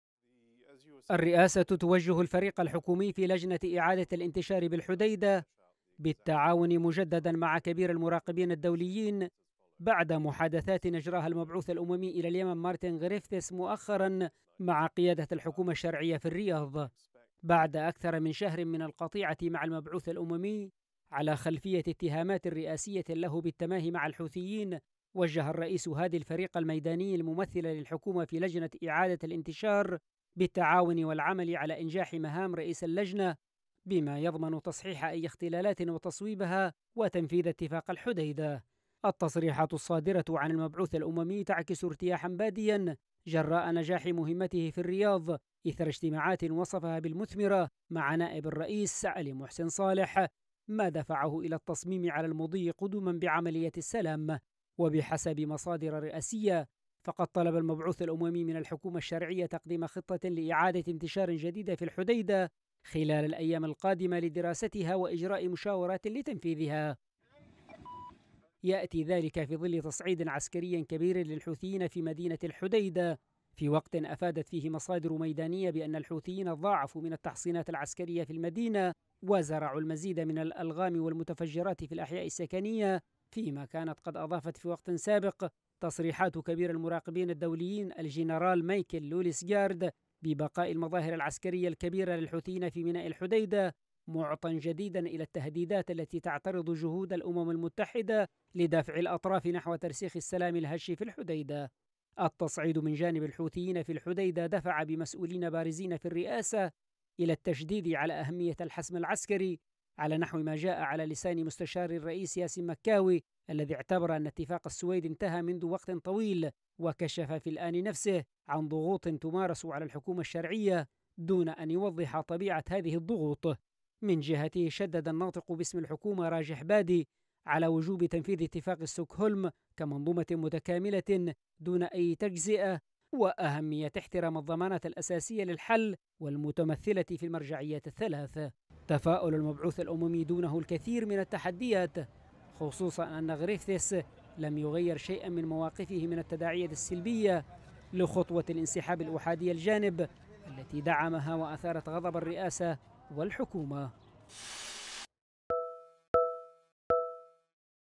الحكومة توجه بالتعاون مع لوليسغارد وقيادات تدعو إلى الحسم العسكري في الحديدة|تقرير:ياسين التميمي
وجهت الرئاسة، ممثليها في لجنة إعادة الانتشار بالتعاون مع كبير المراقبين الدوليين في الحديدة الدنماركي مايكل لوليسغارد.
وقال نائب الرئيس علي محسن الأحمر إن الرئيس هادي وجه ممثلي الحكومة بالعمل على إنجاح مهام رئيس لجنة تنسيق إعادة الانتشار، بما يضمن تصحيح أي اختلالات وتصويبها وتنفيذ اتفاق الحديدة...